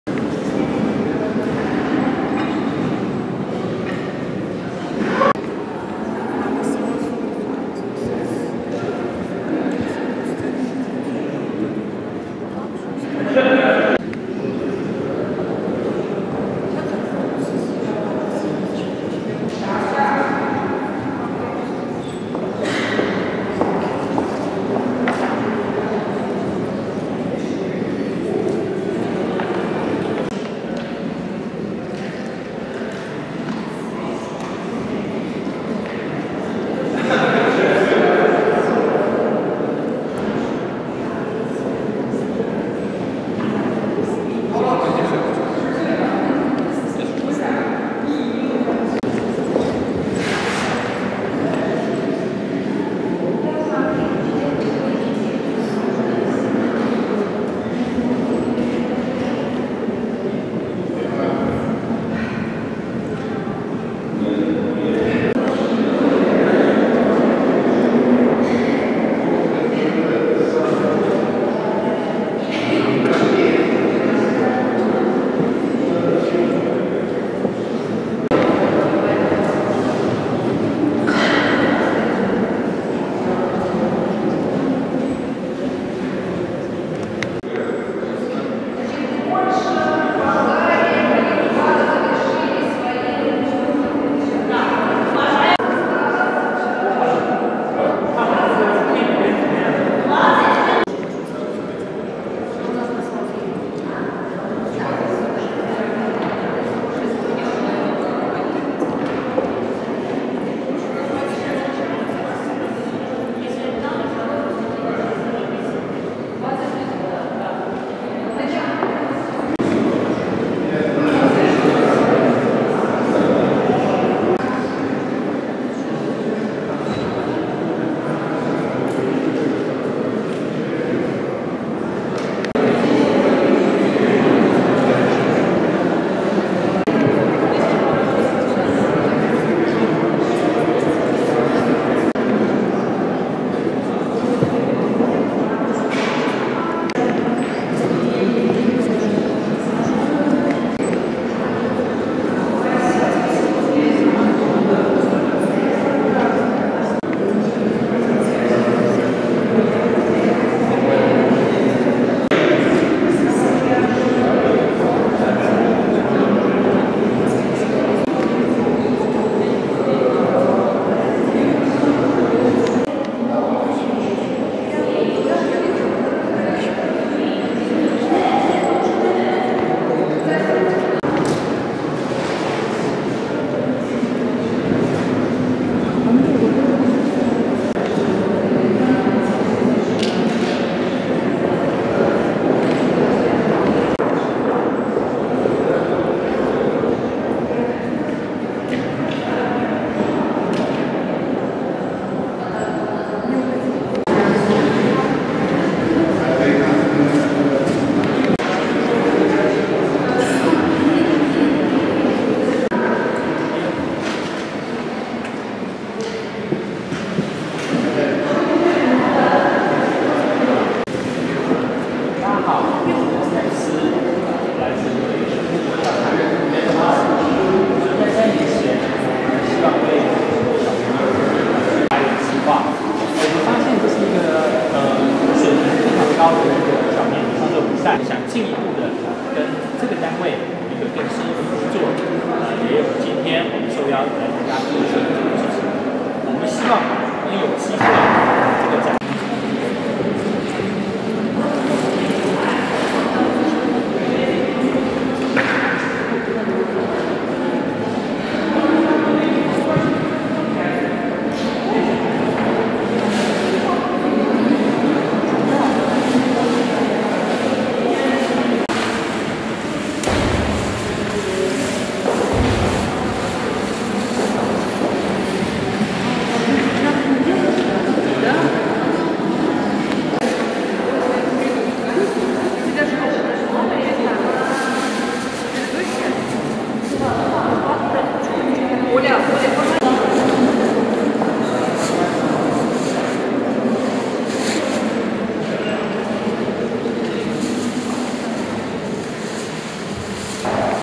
International contest of kid’s drawings backstage - Belarus 2020
15th International Art and Graphics Contest and the 5th International Cartoon and Film-Strip Contest “In My Homeland” took place near Minsk in Smalyavichy city.